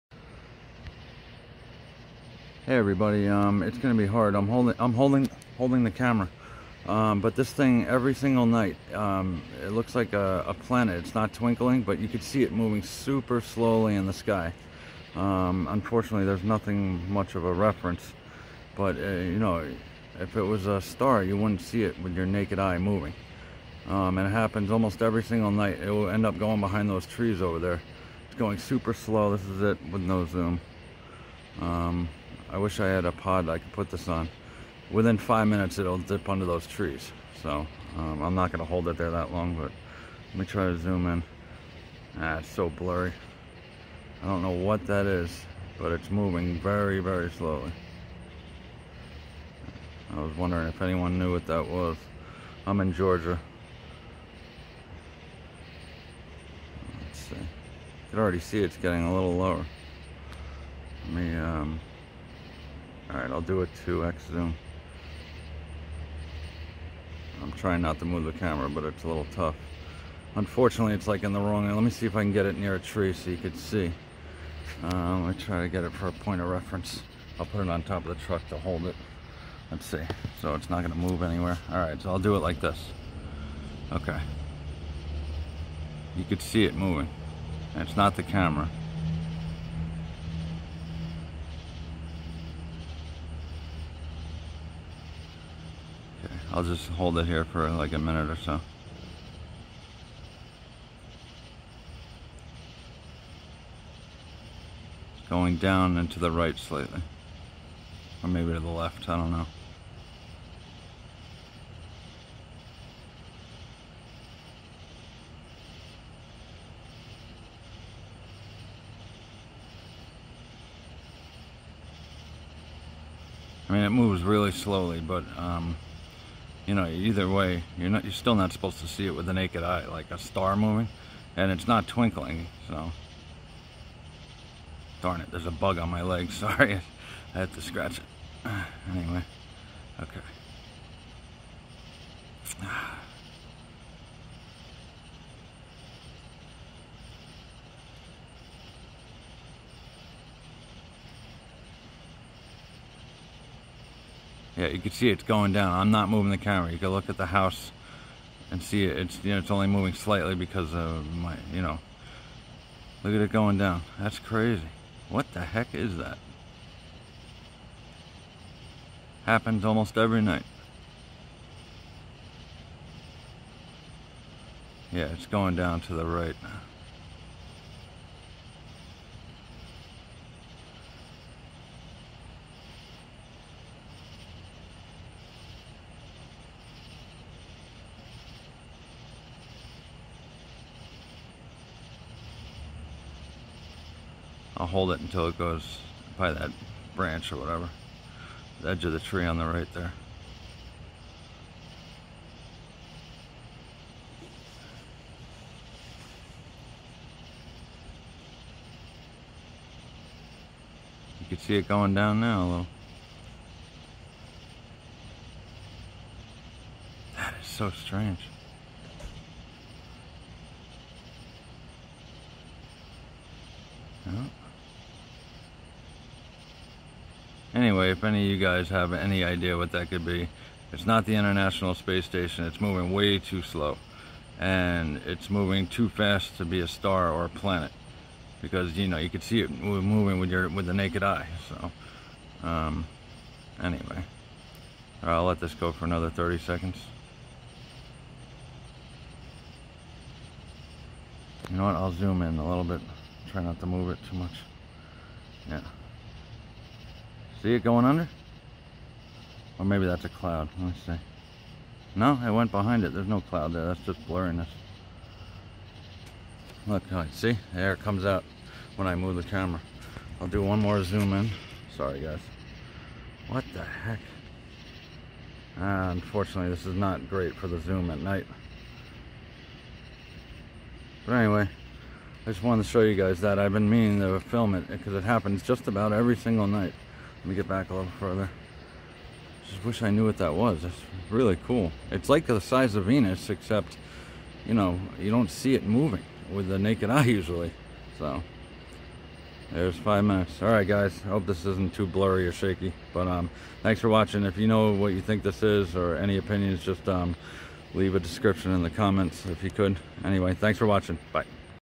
Slow Moving Venus Sized Object In Night Sky With No Other Stars!! What Is This?
Here is a cool slow moving object that looks about the size of Venus in the sky.  There are no other stars or planets visible in the sky!   It's slow,  but still moving quick enough to see it with the naked eye.  That can't be done while looking at planets or stars,  and it's too slow to be the ISS,  a shooting star,  or a satellite.  This happens almost every night!  None of us who have seen it have a clue as to what it is!!  I'm dying to know what it could be?  Any opinions would be greatly appreciated everyone!
-Chris